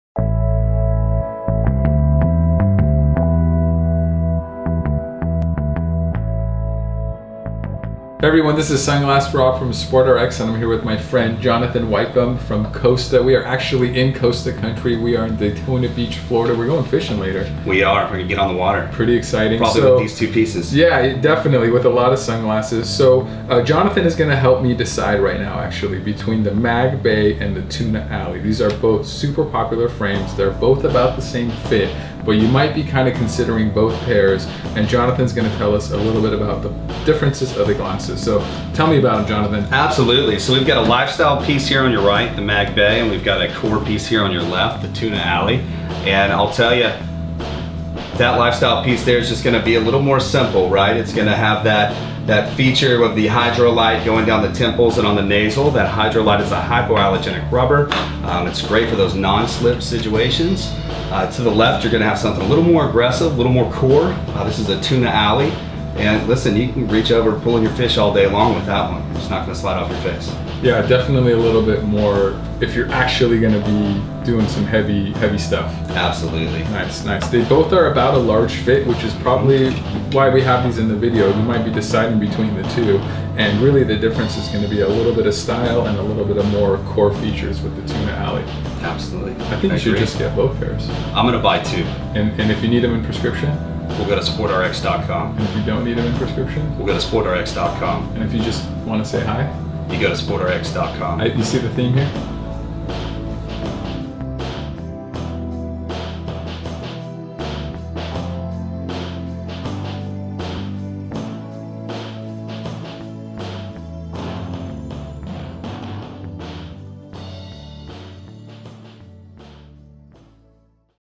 Costa Tuna Alley vs. Mag Bay | SportRx
Deciding on a large fit pair of Costas? Jonathan Weikum from Costa joins Rob from SportRx to do a side-by-side comparison between the Costa Tuna Alley vs Mag Bay. J-Dub and Rob review the key similarities and differences between the two sunglass frames. While one pair has aggressive, core features, the other has more an of a lifestyle look and feel. Which is your favorite?